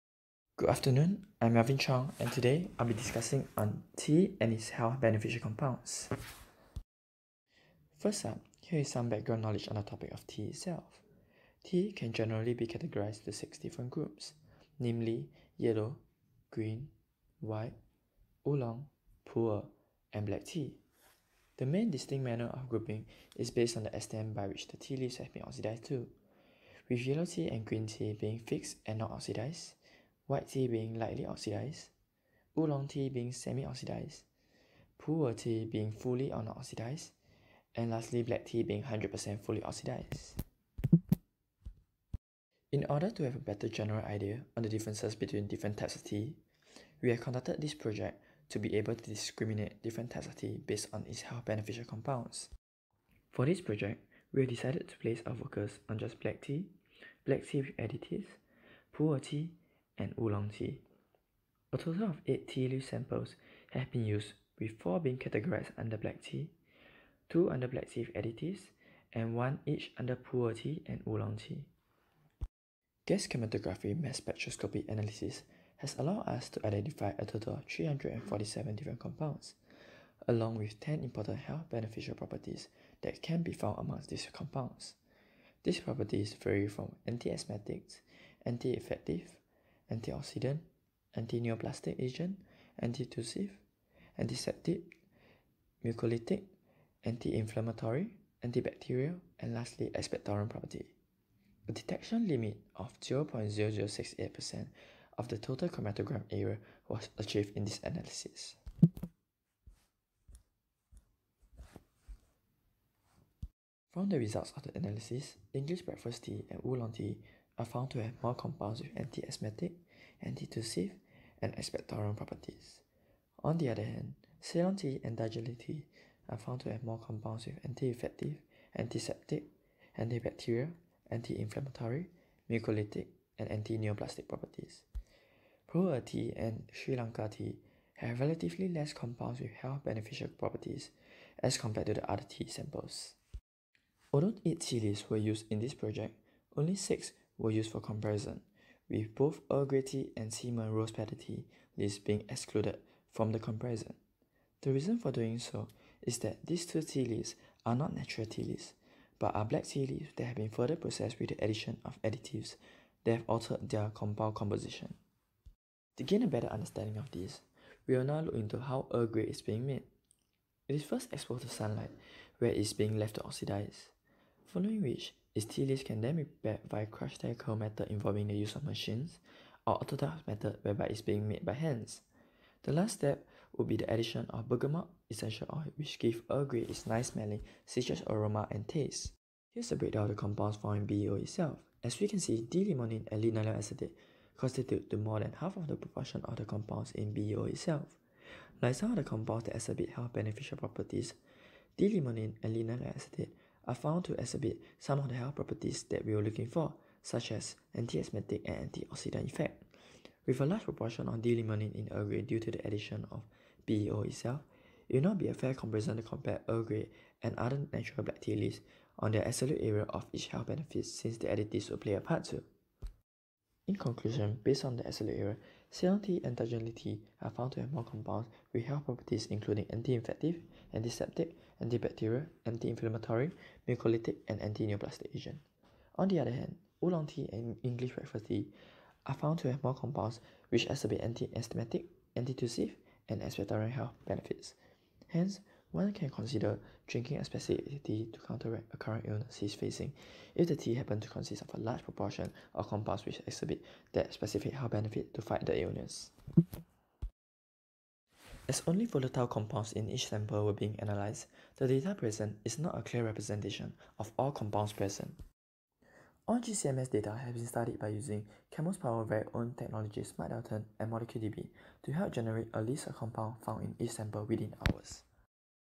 Benefits of Drinking Tea
Tea is often a times used as a beverage between meals, and it is said to have many health benefits. However, do all types of tea bring the same health benefits, will drinking different types of tea helps tackle different kinds of illnesses? Allow us at ChemoPower to take you through this journey to uncover the underlying factor behind these health properties of tea!

茶通常被用作两餐之间的饮料，并且据说拥有许多健康益处。但是实际上所有类型的茶都有相同的健康益处吗？是否喝不同类型的茶有助于缓解各种疾病呢？让ChemoPower为您解答所有疑虑，揭示茶叶这些健康特性背后的潜在因素!